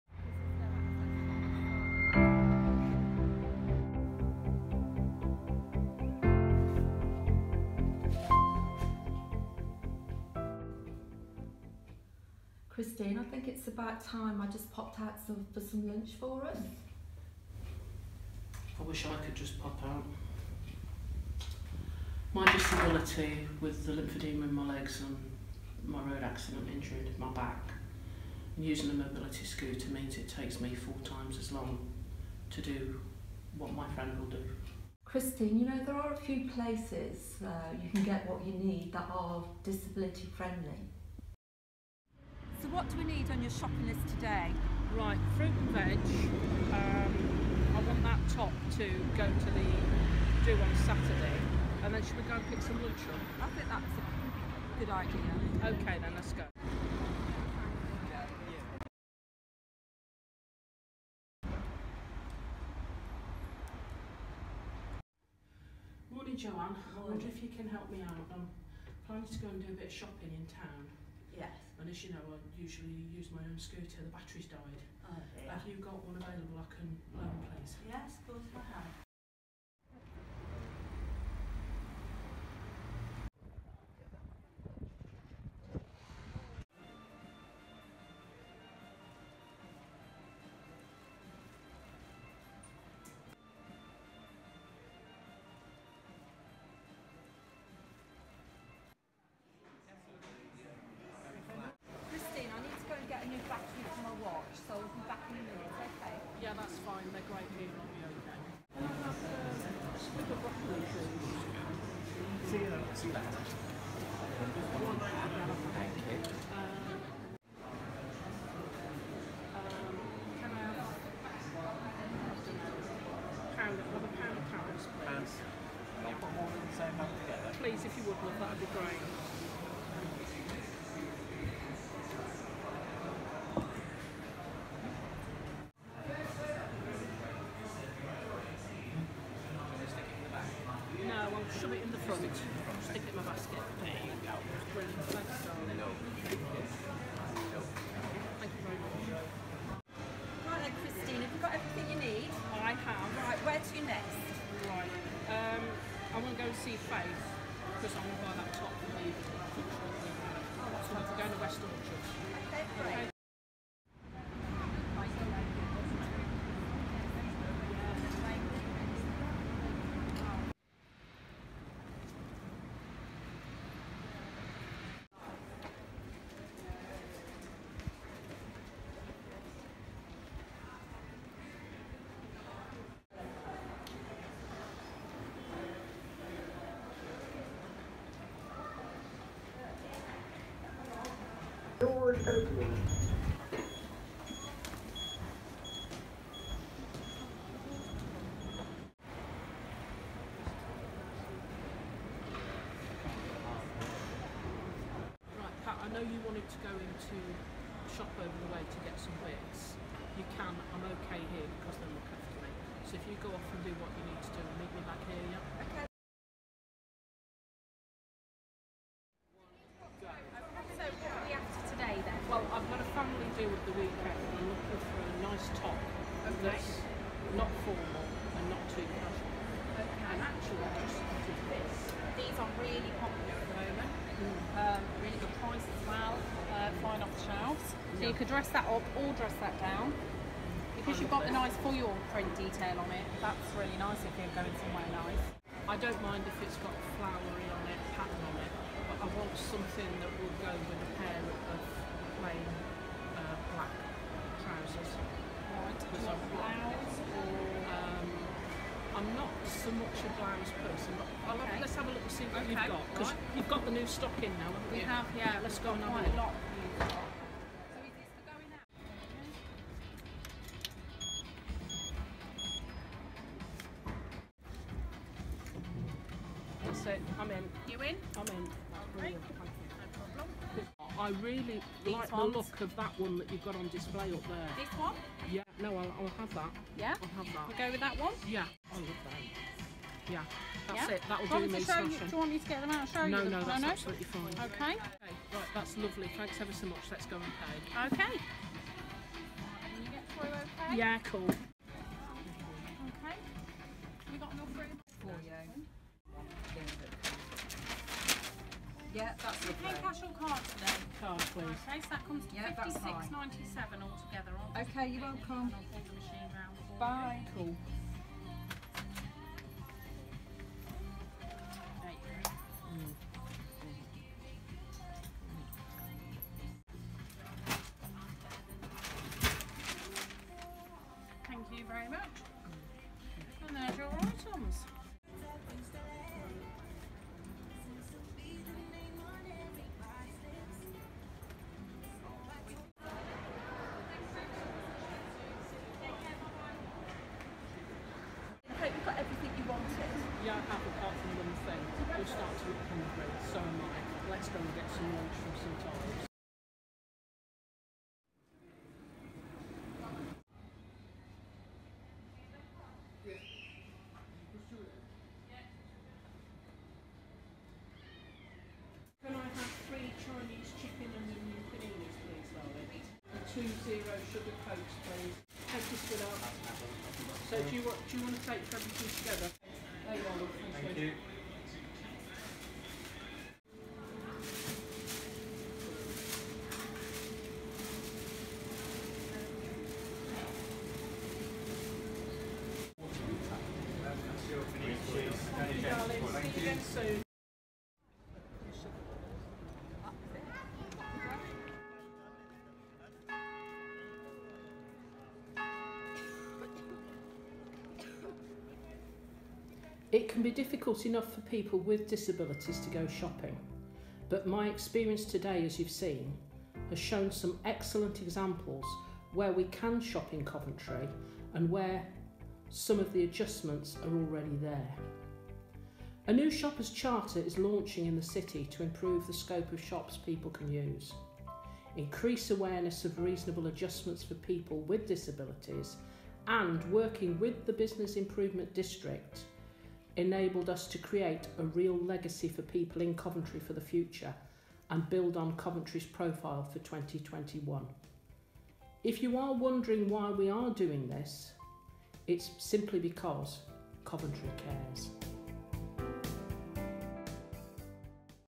Disability Shoppers Charter
A new Disability Shoppers Charter has launched in Coventry to improve access, freedom, and inclusion for people with disabilities, and showing the 'Purple £' as an important source of income.